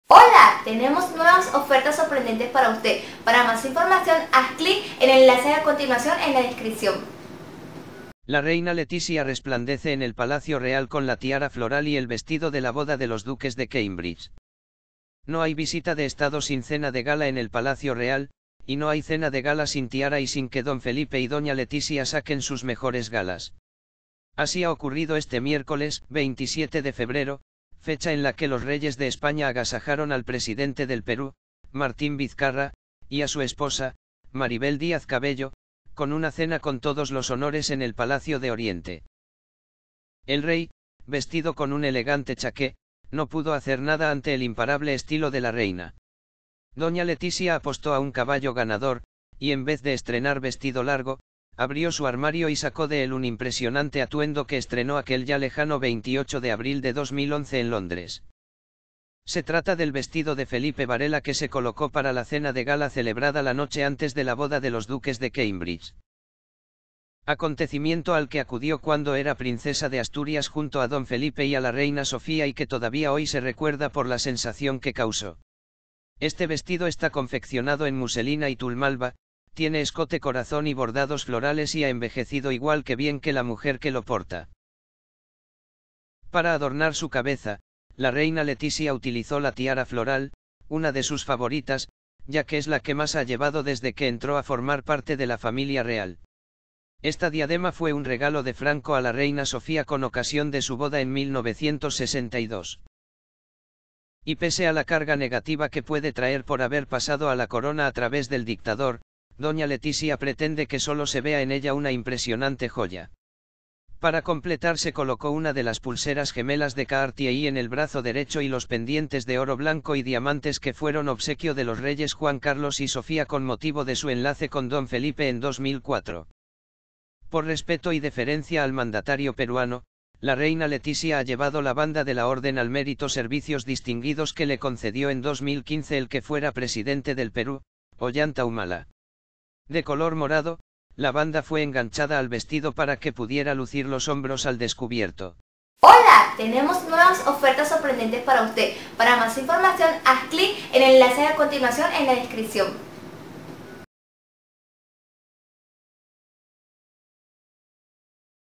La Reina Letizia resplandece en el Palacio Real con la tiara floral y el vestido
La Reina Letizia resplandece en el Palacio Real con la tiara floral y el vestido de la boda de los Duques de Cambridge

Doña Letizia sacó del armario el Varela que se enfundó en la cena de gala previa a la boda del Príncipe Guillermo y Kate Middleton en 2011.